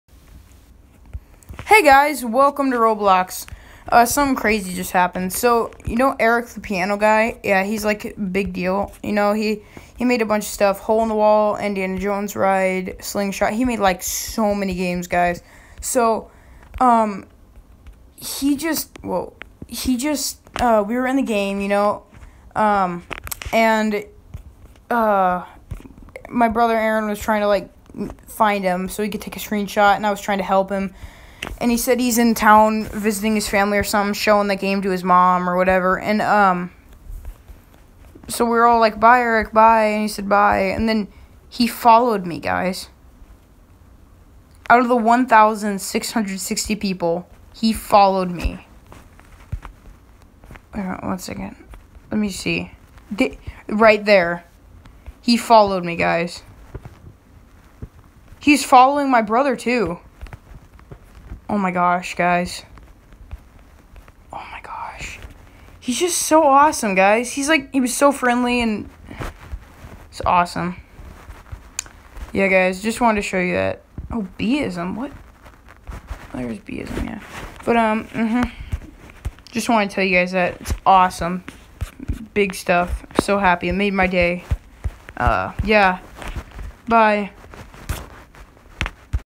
ERICTHEPIANOGUY FOLLOWED ME!
OMG GUYS IM SO HAPPY RIGHT NOW. THIS IS NOT FAKE CHECK MY FOLLOWERS OMG.